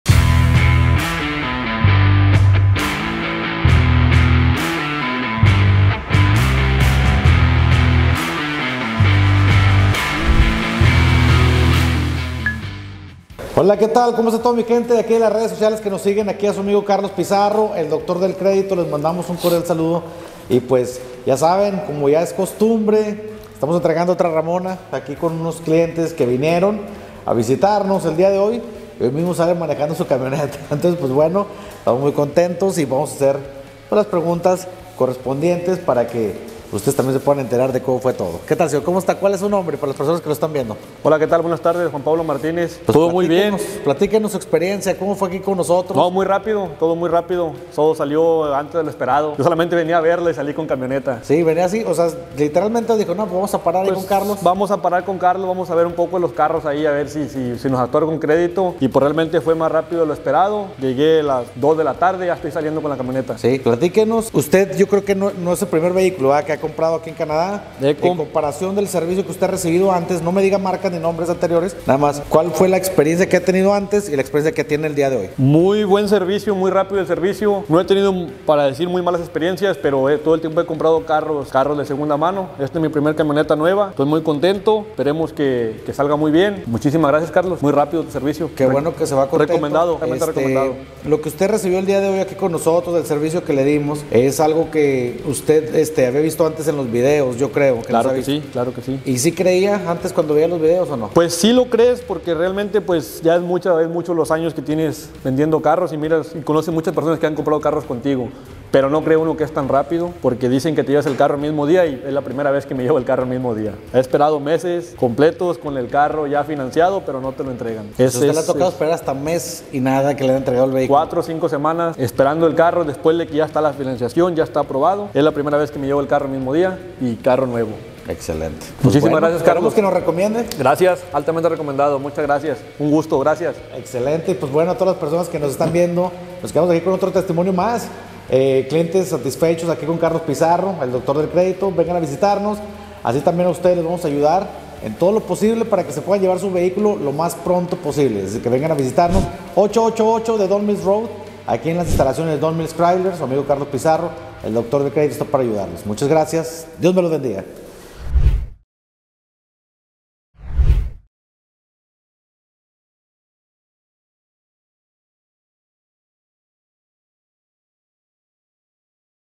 Todo se dio antes de lo esperado - 2023 RAM 1500 Classic - Doctor del Credito
Ven por tu 2️⃣0️⃣2️⃣3️⃣ RAM 1500 Classic 🛻

Todo muy bien y rápido. Todo salió antes de lo esperado. Solamente venía a ver y salí con carro. Llegué a las 2:00 p.m. y hoy mismo le llevo la camioneta. Esta es mi primer camioneta nueva. Altamente recomendado el servicio. Cuando veía los videos de Carlos Pizarro el doctor del credito, no creía que fuera tan rápido. En otros lugares tuve que esperar más de un mes para que me entregaran el vehículo y aquí fue el mismo día. 🇲🇽📍🇨🇦